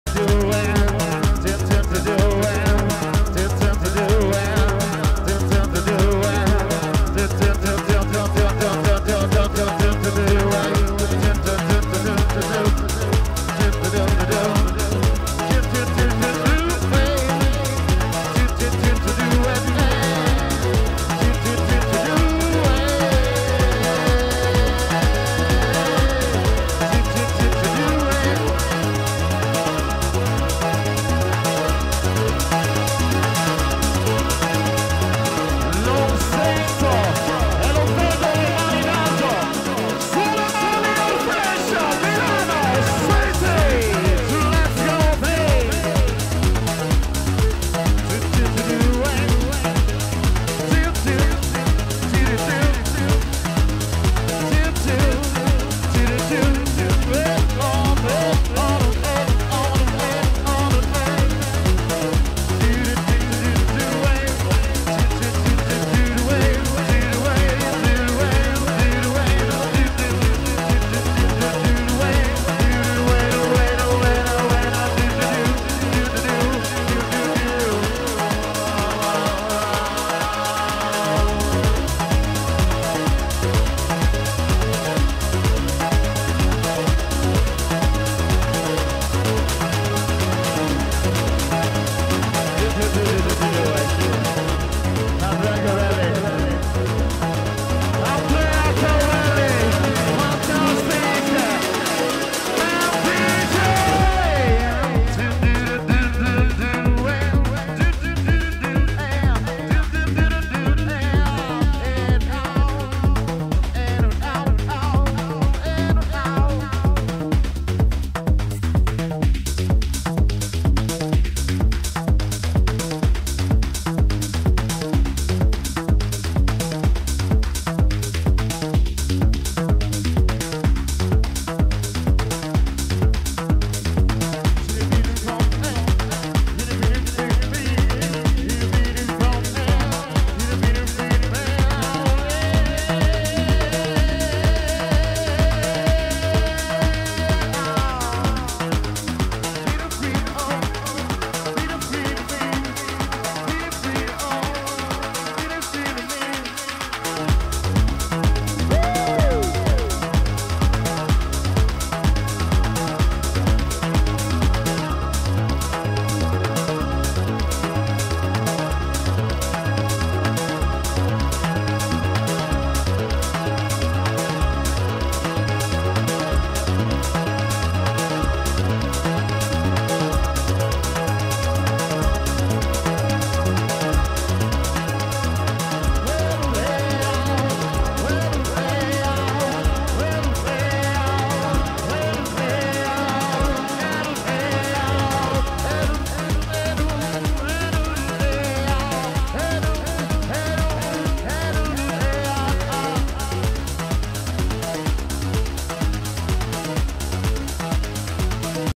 Andrea Corelli live @ Old Fashion Cafe (Milano) 18-04-2009
Andrea Corelli Live DJ Set during Elite Night - saturday 18th of April 2009 at Old Fashion Cafe (Milano)
Roby SKF on the Voice.
Gianmarco Ferlisi DJ prepairing his gear,
Video shooting by Stefanino (Thank you!!!)